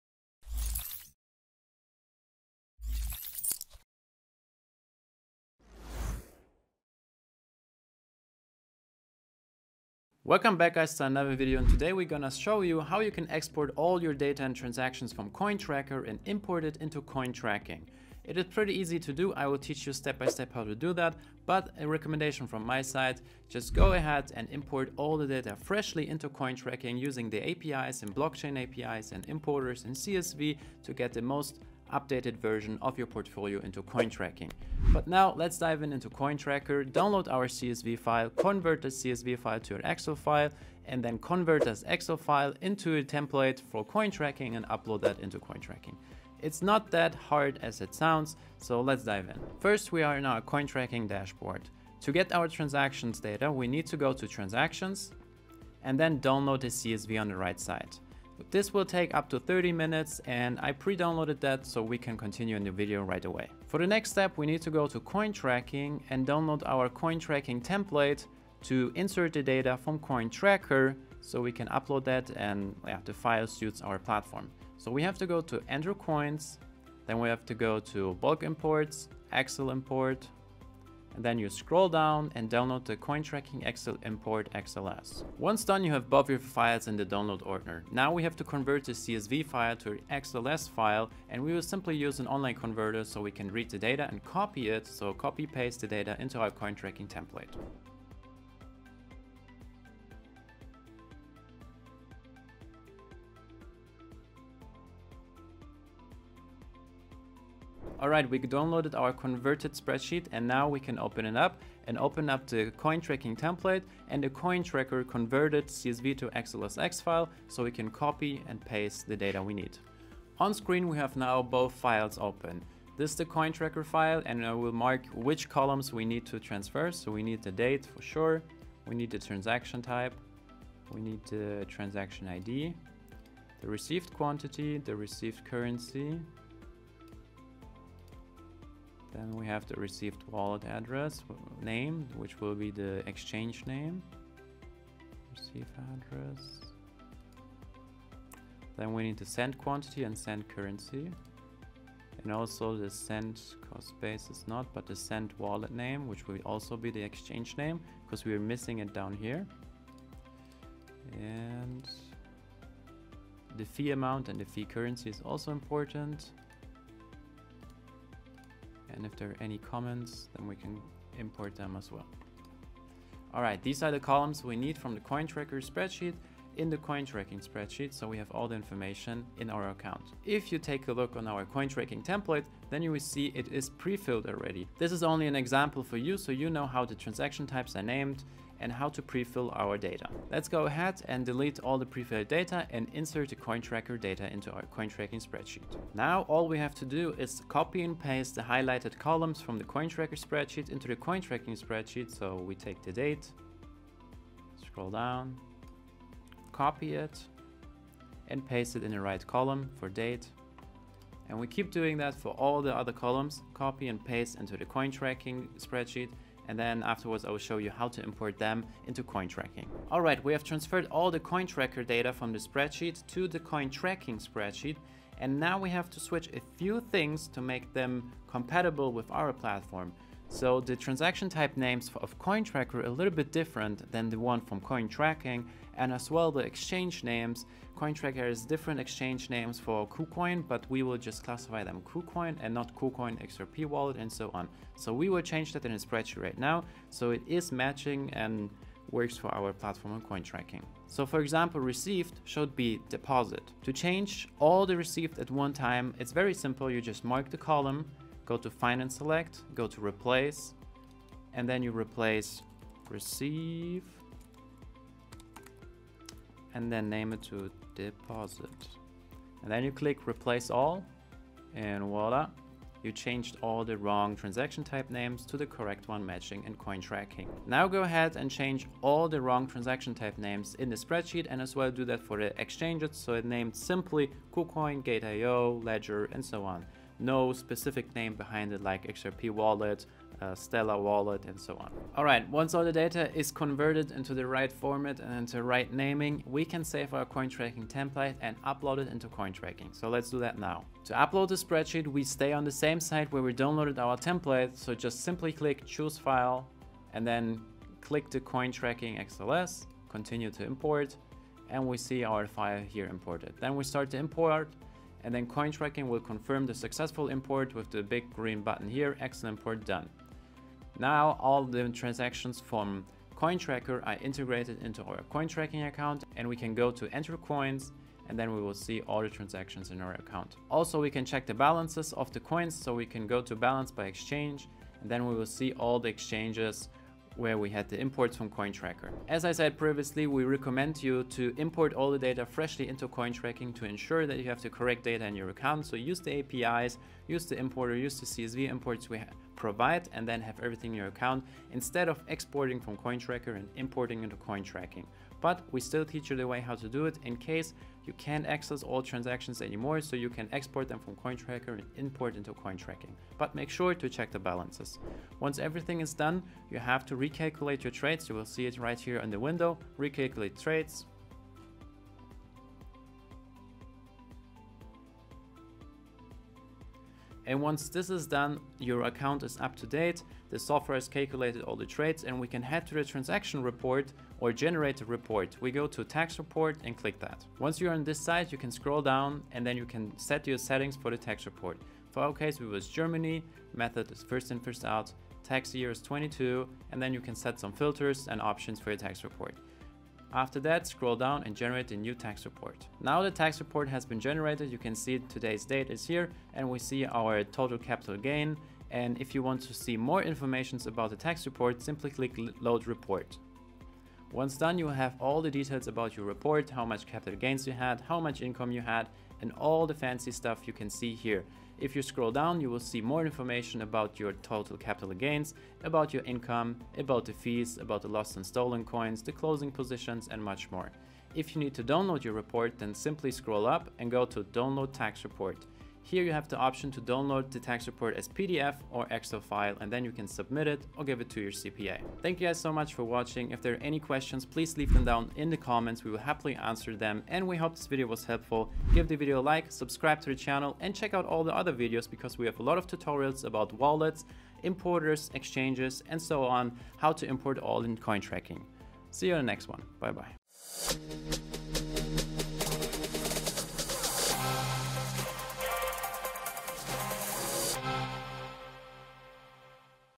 How to Move from CoinTracker to CoinTracking: A Step-by-Step Guide
In this tutorial, we'll show you how to quickly and easily move all your data and crypto transactions from CoinTracker to CoinTracking. Step by step, we'll guide you through the whole process and you'll learn how to:

📌 download your data from CoinTracker
📌 convert files for compatibility with CoinTracking
📌 upload data to CoinTracking
📌 generate and download a complete crypto tax report.

Timestamps:
00:00 Introduction
00:23 - How to download transaction data from CoinTracker
01:03 - How to get the CoinTracking template
01:25 - How to review and adjust data
05:00 - How to upload crypto transaction data to CoinTracking
05:25 - Checking the balances
06:27 - How to generate a crypto tax report on CoinTracking

Stay informed and empowered in the world of cryptocurrencies!